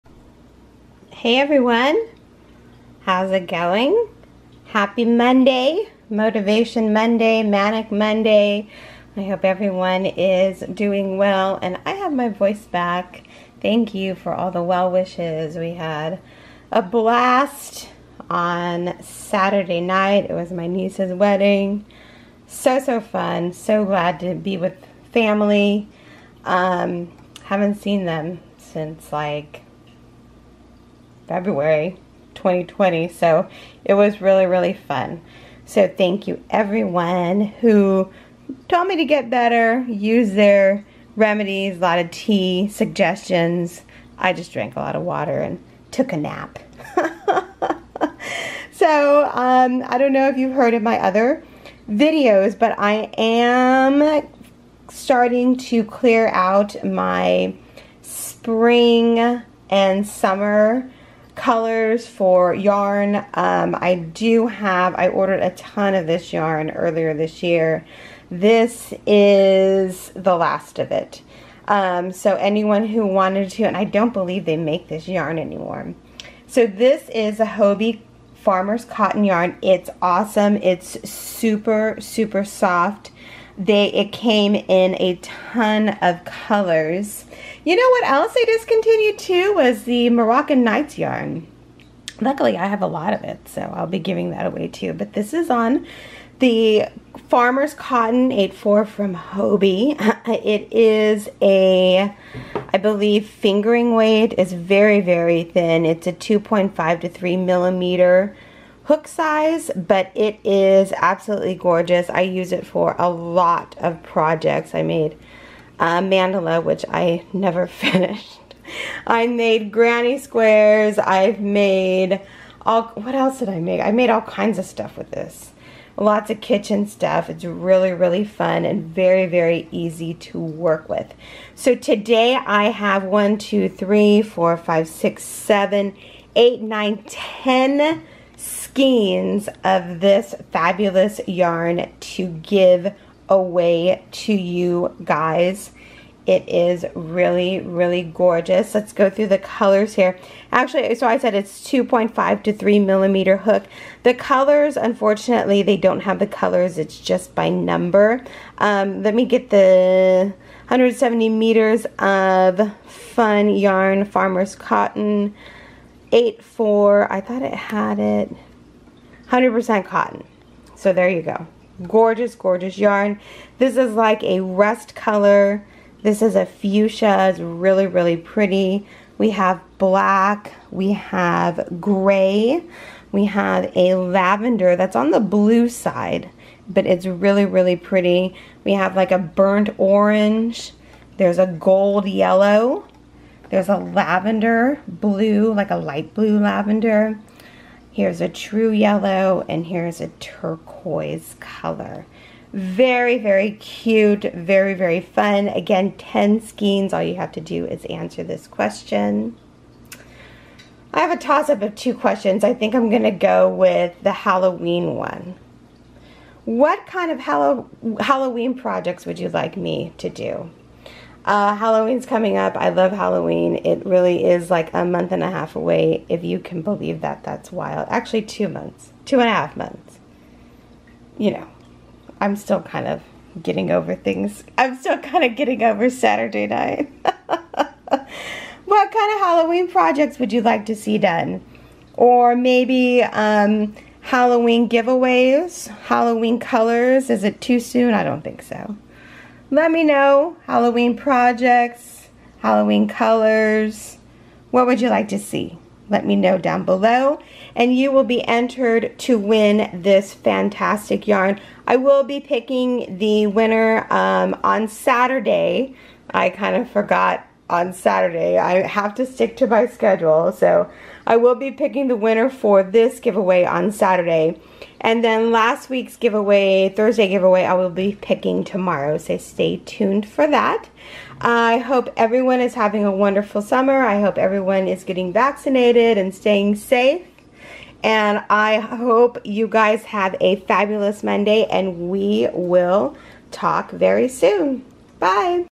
🔴🔴Ended🔴🔴Dazola Designs Free Yarn Giveaway! 10 Skeins of Hobbii Farmers Cotton Yarn!
Hey everyone!  I’m cleaning out my shelves of summer colors  yarn to make room for fall colors yarn!  Woo Hoo!  This giveaway is for 10 gorgeous skeins of the hobbii farmesr cotton yarn!  All you have to do to be entered is answer the question I ask in the video!  Good luck!

The winner of this giveaway will be picked on Saturday, August 21st